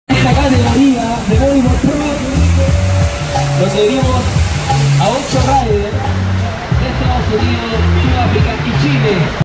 Diego BodyBoard Pro Prone Semi Division at Mall Sport FlowRider Flow Barrel 2024(4)
Flowboarders from around the world traveled to Santiago, Chile for the 2024 FlowRider Flow Barrel contest held by La Ola Mall Sport in Condes, Region Metropolitana. Mall Sport is a retail shopping center dedicated entirely to sporting goods and activities with a 4 pump FlowRider / Waveloch FlowBarrel.